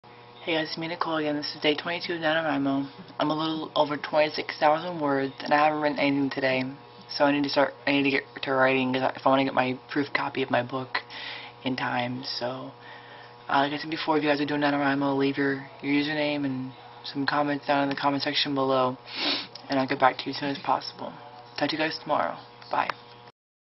NaNoWriMo 2010 - Day Twenty - Two
Day twenty - two of NaNoWriMo.  Word count as of 9:54 p.m. is 64,138.